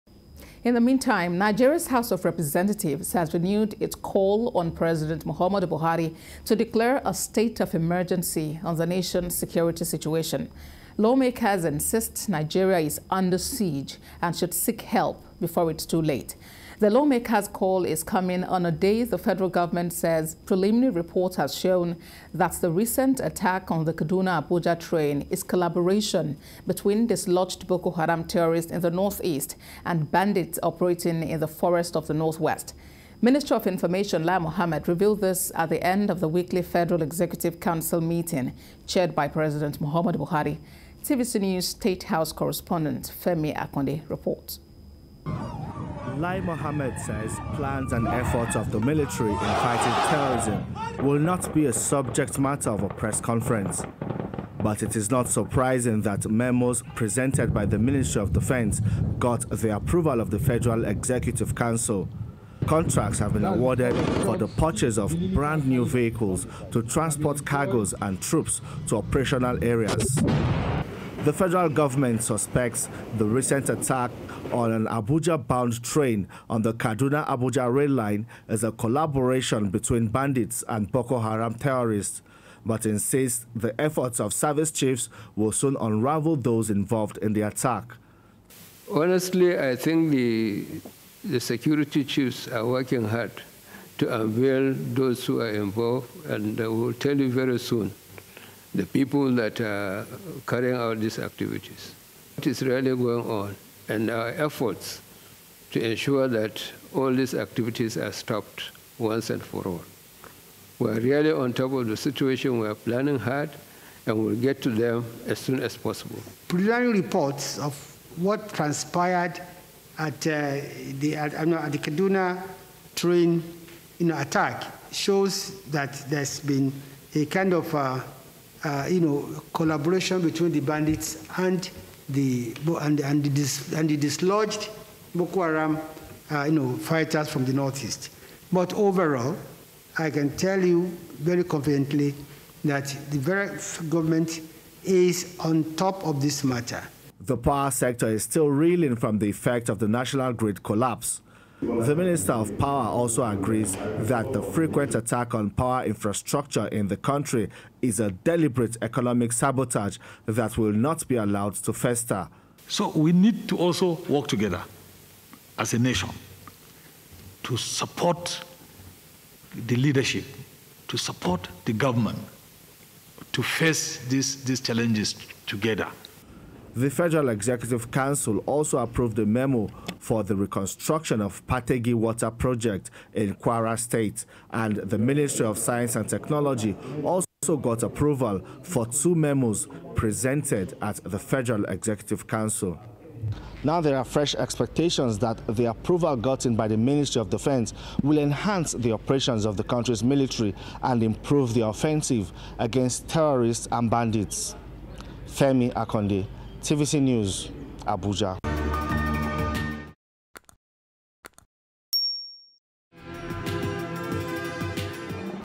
FEC Approves Contracts For Purchase Of Operational Vehicles
The Federal Executive Council (FEC) Contracts For Purchase Of Operational Vehicles